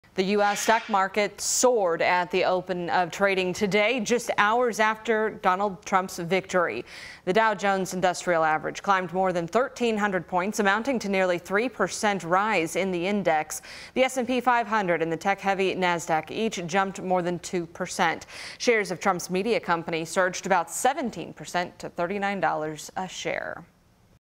Stock markets surge after Trump declared president
The expectation is that a victory by Trump will lead to faster economic growth and more market-friendly policies.